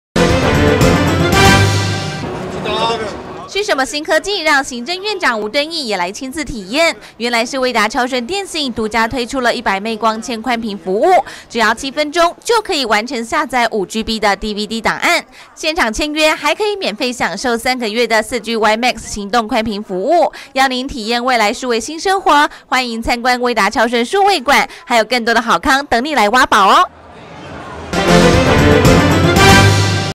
威達數位-快訊12111w.wmv
行政院長吳敦義參觀2009台中資訊展，對於威達超舜電信推出獨家的100M光纖寬頻網路應用讚不絕口！